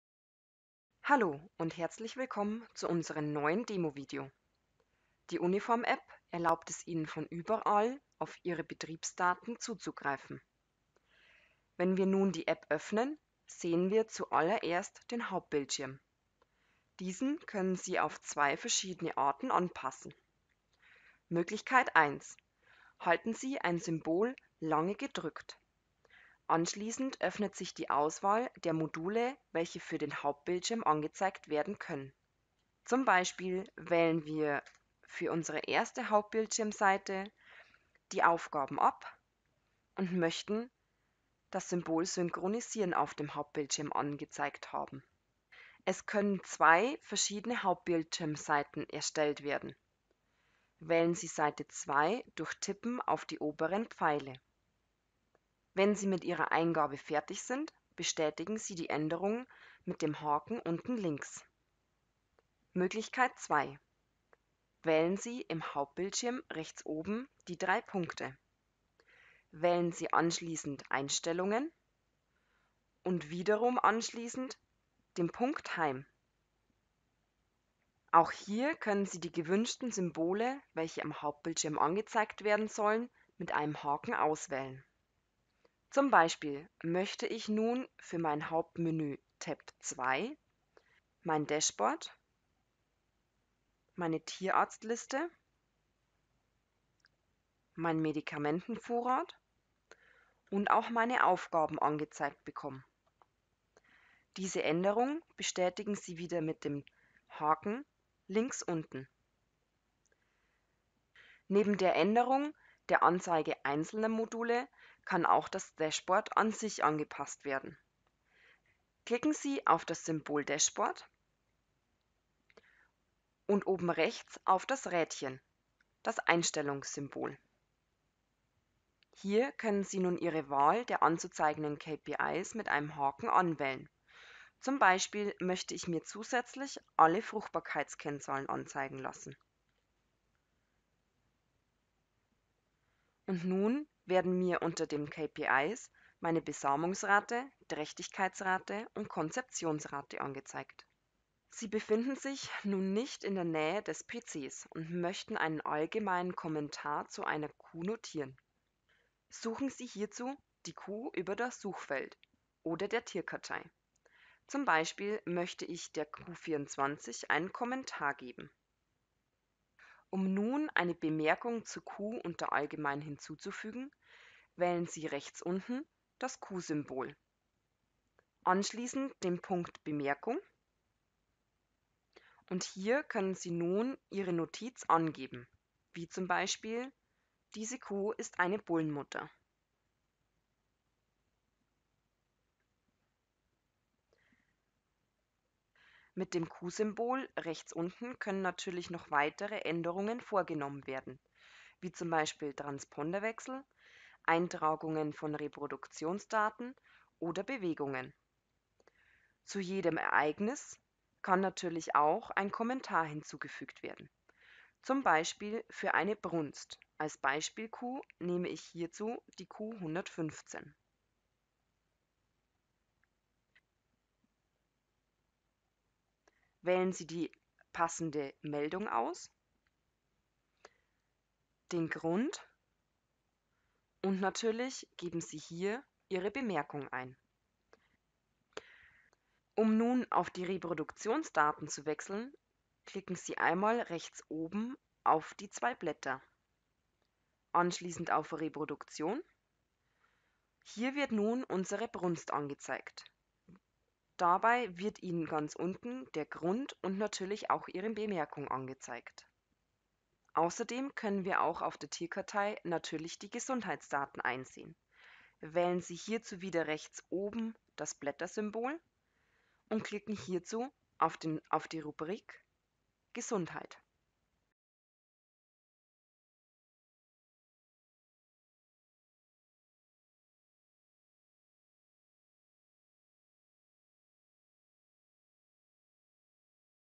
Demovideo Tipps zur Verwendung der UNIFORM-App
In unserem neusten Demovideo möchten wir Ihnen einige Tipps zur Verbesserung der Anwendung mit unserer App zeigen. Hier finden Sie Hinweise zum Hinzufügen eines Kommentars zu einer Kuh, zum Finden von Gesundheitsdaten und zur neuen Anordnung Ihres Dashboards.